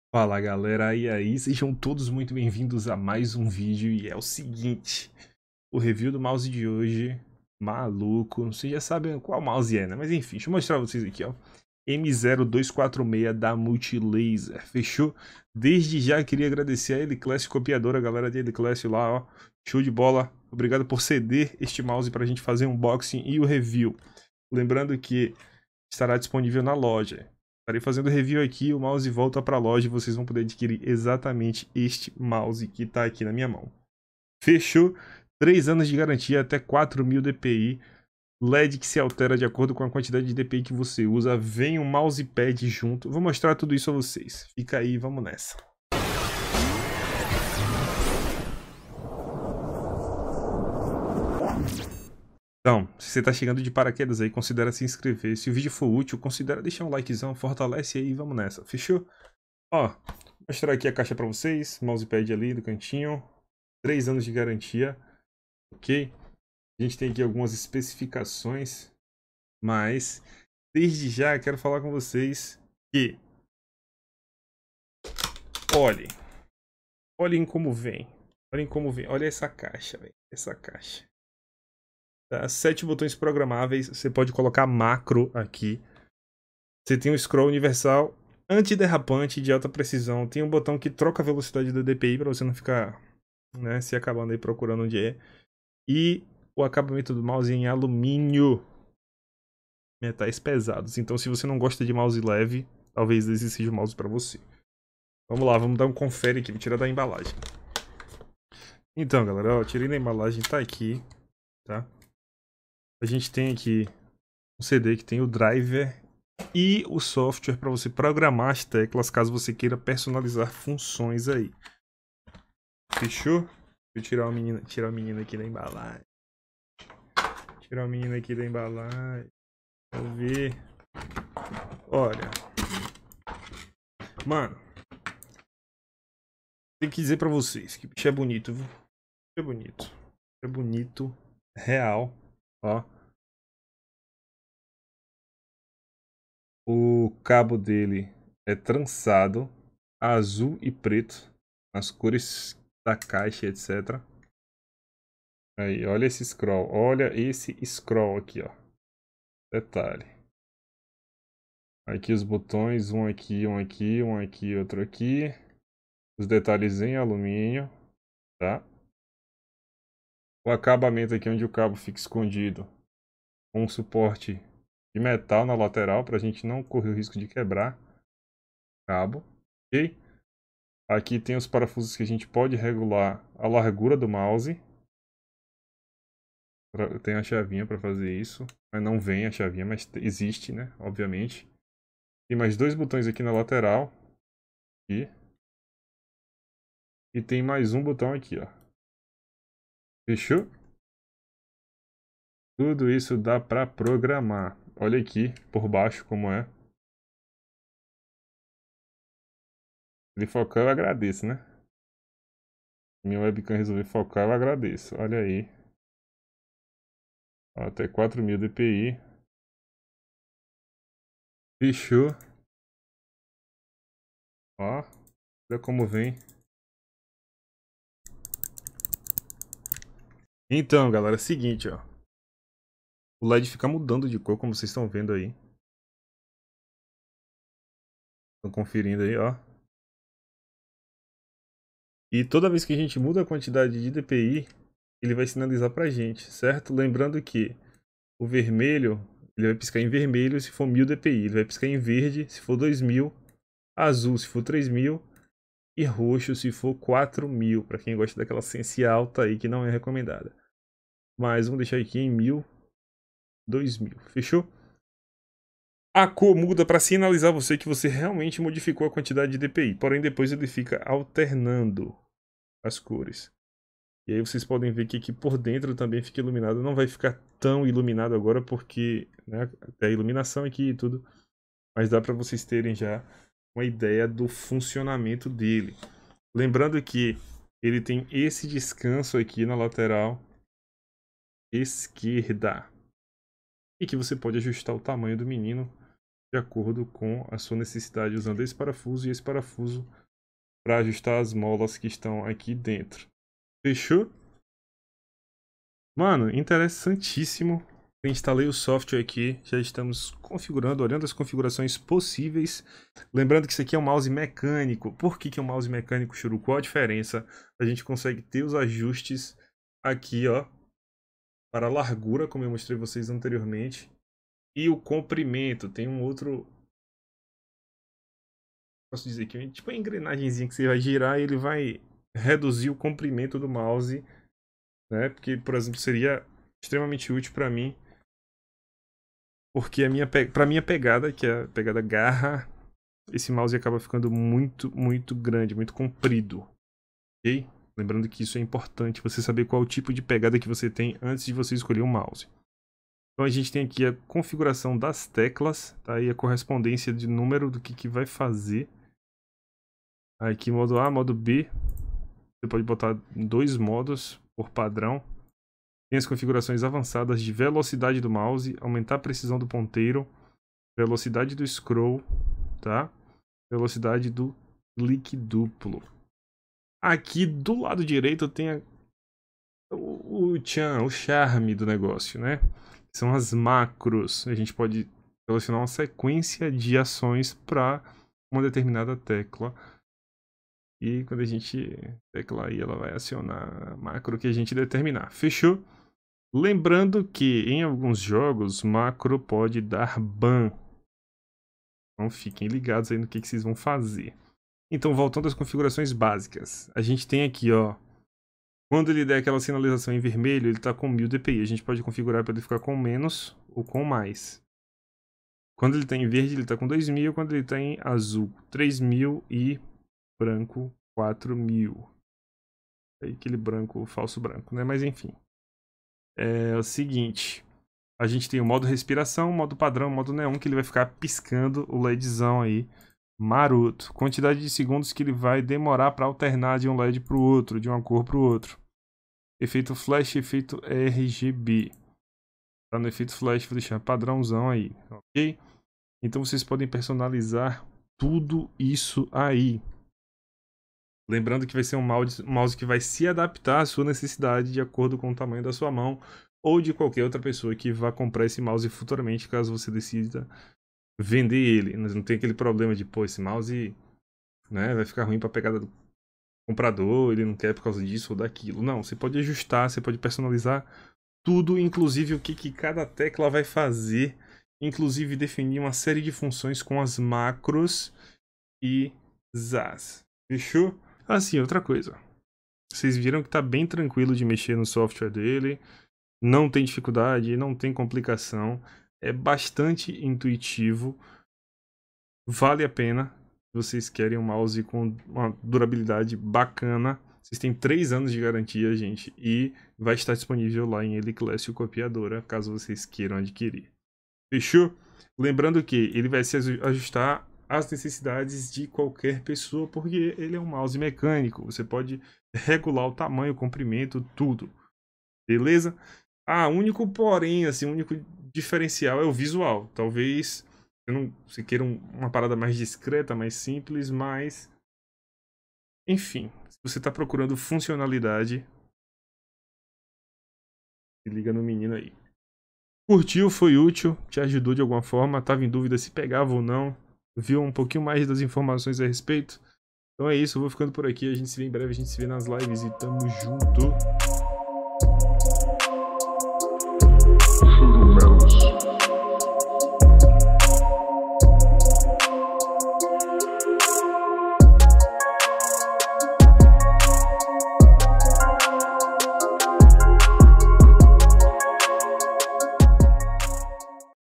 [Unboxing] Mouse Gamer Mecânico (Warrior MO246 da Multilaser)
Hoje a review é de um Mouse Gamer que pode fazer toda diferença na hora de jogar, com 4 velocidades de DPI setadas de fábrica, superfície emborrachada e suporte a Macro.

Fácil instalação, conta com o sistema de Plug & Play e é compativel com Windows 2000 ou Superior. Com um Design Anatômico e a pegada firme, você terá muito mais precisão ao atirar, se locomover e mirar!

Características:
● Superfície Emborrachada ;
● Plug & Play ;
● Suporta Macro ;
● Possui Software para Controle de Funções;
● Peso: 0,151 Kg;
● Taxa de Frequência: 500Hz ;
● Tempo de Resposta: 1Ms;
● Configuração de Macros Através De Software;
● 4 Padrões de DPI configuráveis: De 250 até 4.000
● Cabo de Nylon com 1,5M de Comprimento.

Ponto negativo:
● O conector USB NÃO É Banhado a Ouro (apesar da embalagem afirmar que sim).

🎮⬇ Conecte-se comigo! ⬇🎮

Tamo junto! 💜
Schurumelos.